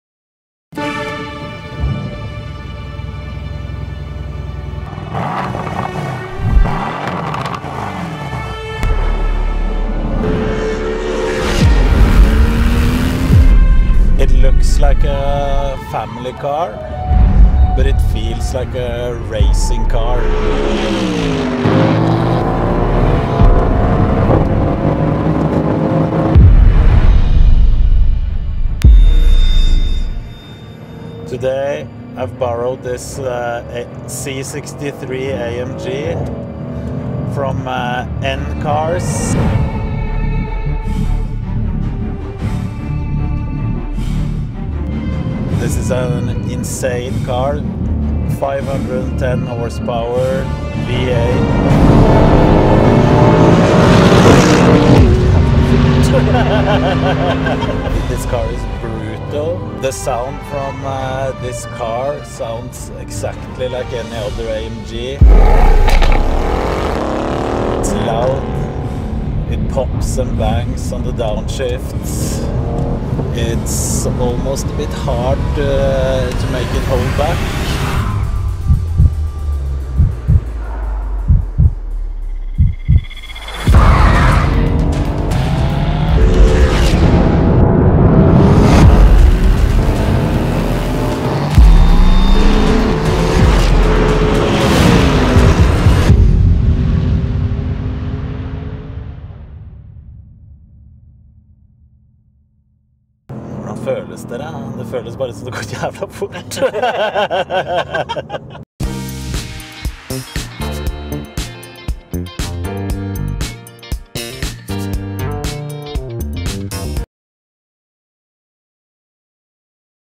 KNUT DRIVING CRAZY FAST AMG!? Ft. Mercedes C63 AMG!!
Thank you N-cars for letting me borrow this c63 Mercedes AMG for a day!
This was a crazy experience and I hope you would like it too!